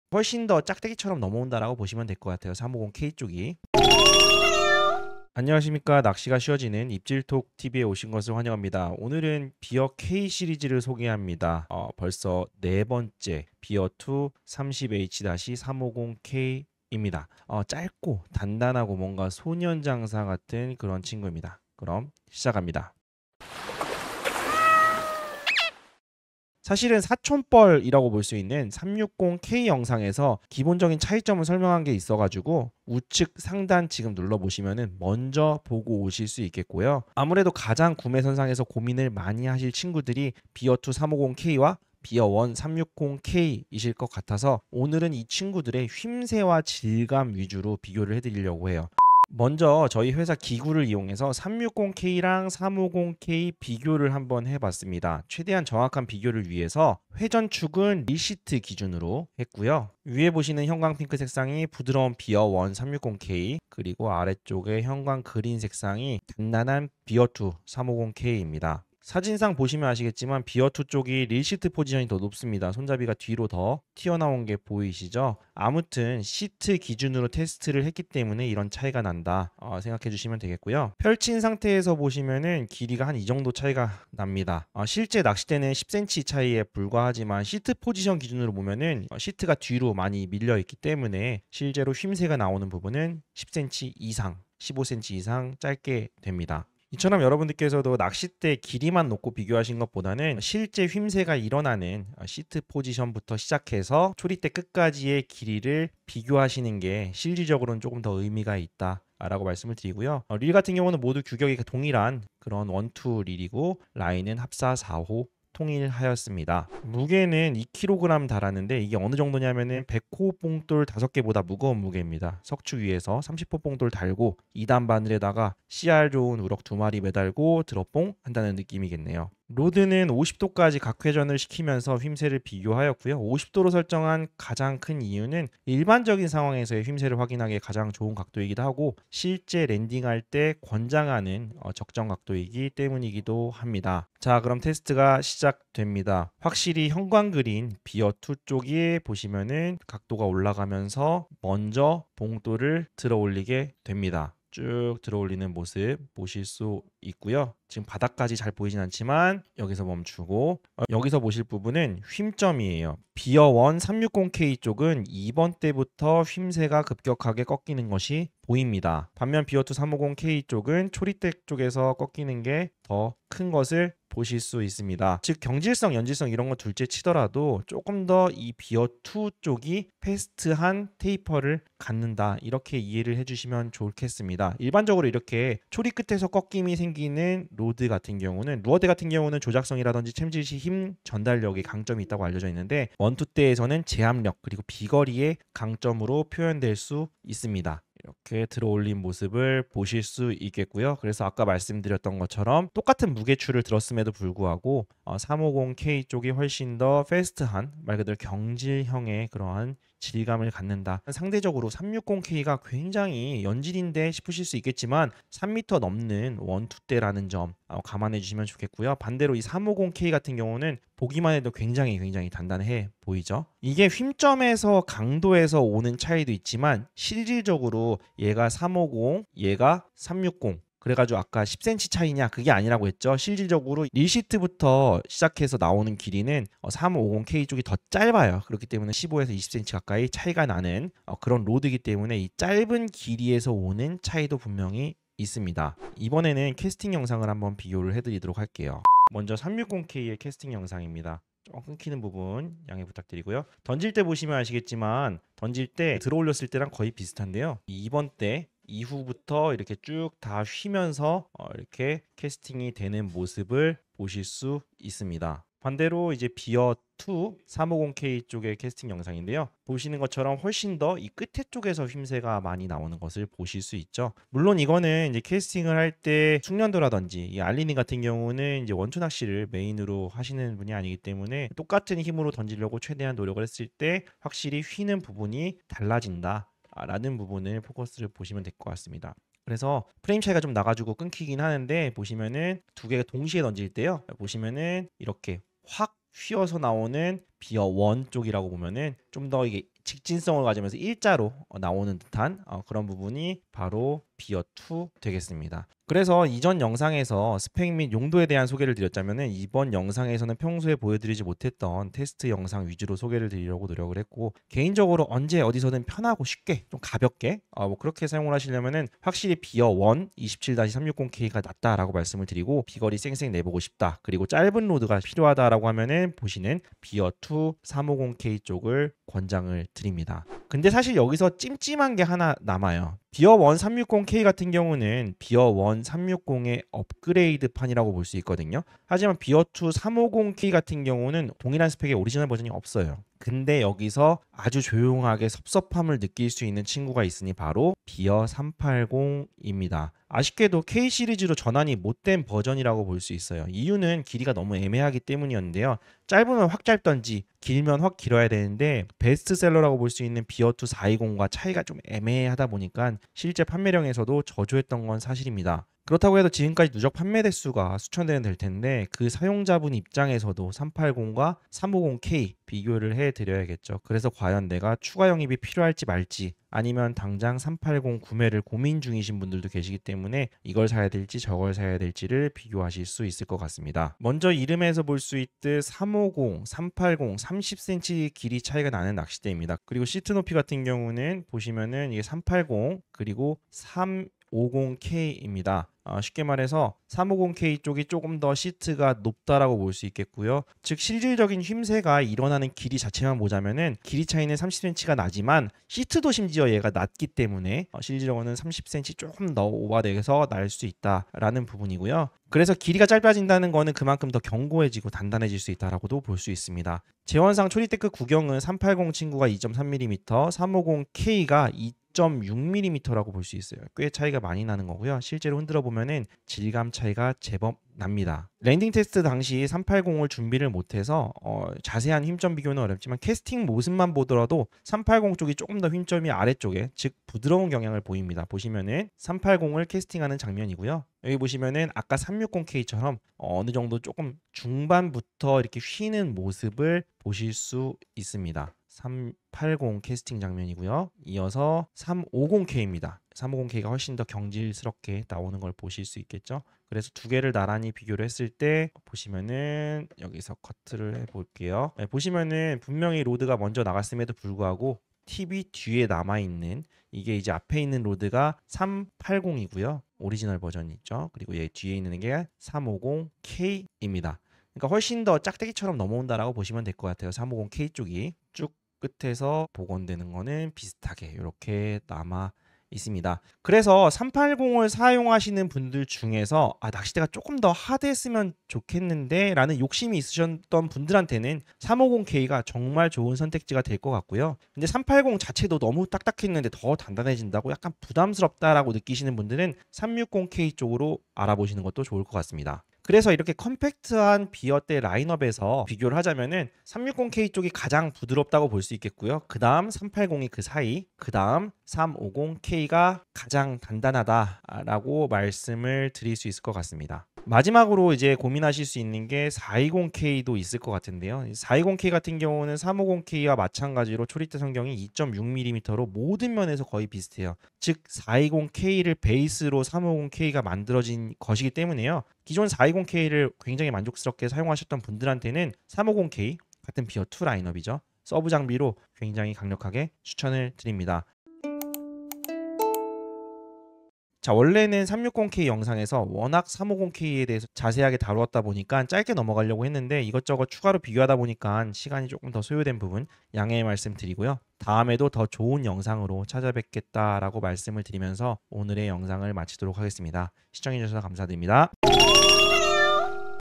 [원투대 리뷰 #18] 원투낚시대 유정비어2 30H-350K 공식 리뷰영상. 물건은 물건이네~
비어2 30H-350K 리뷰입니다.

피드백 최대한 참고하여 휨새영상도 촬영하여 첨부합니다.

*구독버튼 누르면
더 많은 낚시정보를 받아보실 수 있습니다.

유정낚시 쇼핑몰에서는
다양한 낚시용품들이 가득